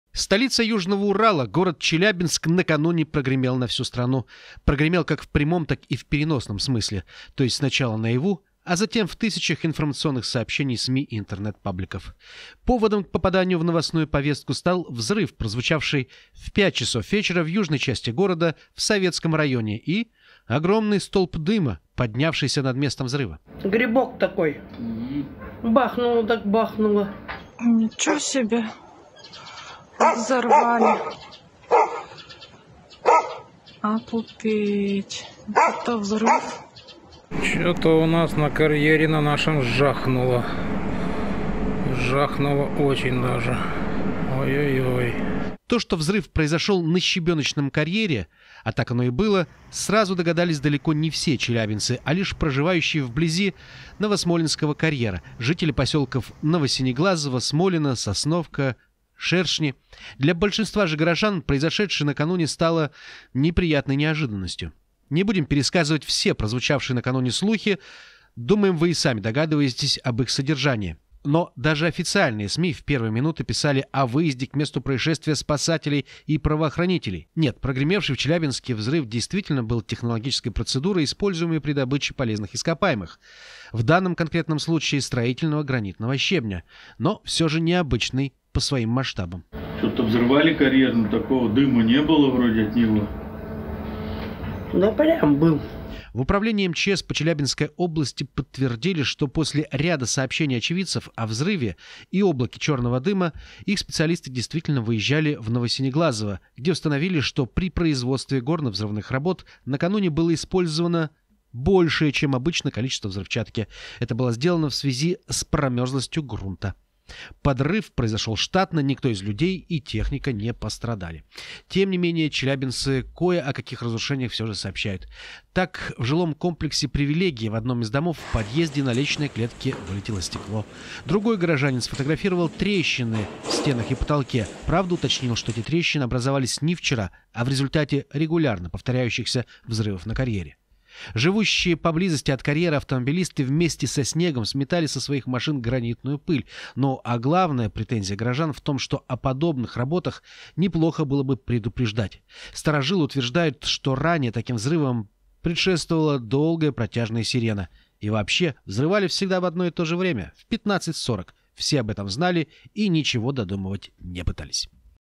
В Челябинске что-то бабахнуло!
Накануне прямо в черте Челябинска прогремел взрыв, который почуствовали во всех районах города.